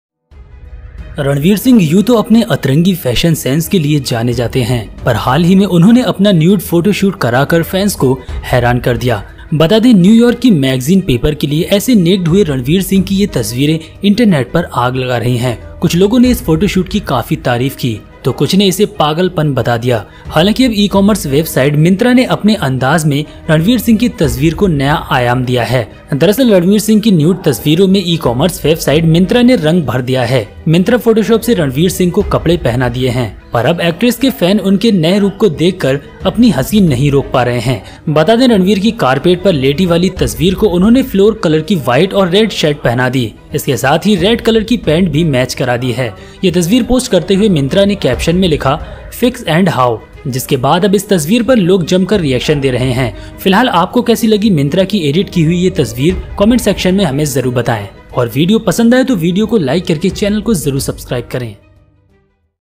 Mytra Recreate Ranveer Singh Naked Photos and Wear Clothes | Ranveer Singh Nude Photos | Bollywood
Mytra Recreate Ranveer Singh Naked Photos and Wear Clothes | Ranveer Singh Nude Photos | Bollywood News
Ranveer Singh is known for his unusual fashion sense. But recently she surprised the fans by doing her nude photoshoot. Let us tell you that these pictures of Ranveer Singh, who got naked for New York's magazine Paper, are setting the internet on fire. Some people praised this photoshoot a lot and some called it madness. However, now the e-commerce website Mantra has given a new dimension to the picture of Ranveer Singh in its own style.
Actually, the e-commerce website Mantra has filled the nude pictures of Ranveer Singh. Myntra has given clothes to Ranveer Singh with Photoshop. But now the fans of the actors are unable to stop laughing after seeing their new look. Let us tell you that he wore a white and red shirt of floral color to the picture of Ranveer lying on the carpet. With this, red color pants have also been matched. Posting this picture, Mantra wrote in the caption, 'Fix and How'. After which now people are reacting fiercely to this picture.